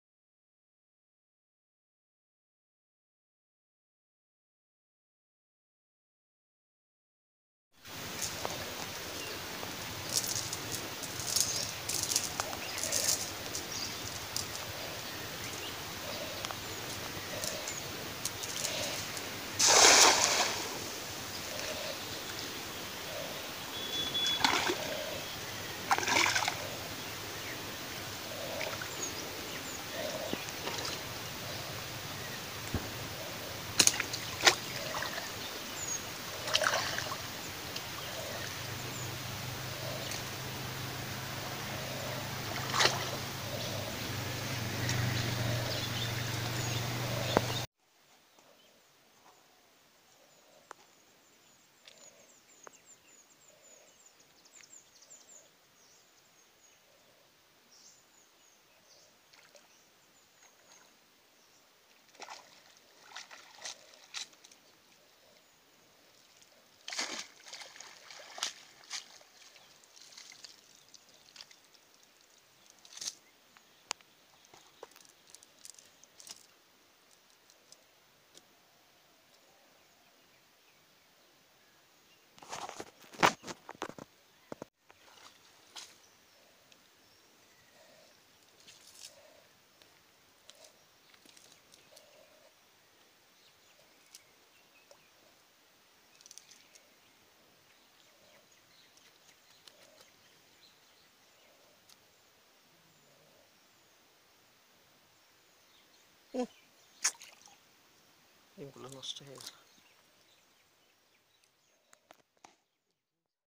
FISHING AT SUNDARBAN 2018
In a space when rivers meet the sea, and the hungry tides wash the lands in continuous rhythm, fishing seems to be the only alternative to survive in the hostile landscape that creates what today is known as the Sundarbans. But when tradition meets modernity, and the rule of conservation overcome the rituals and the customs of the forest dwellers, a conflict between people and state, human and animal arise in a very violent way in one of the most complex landscape of the world.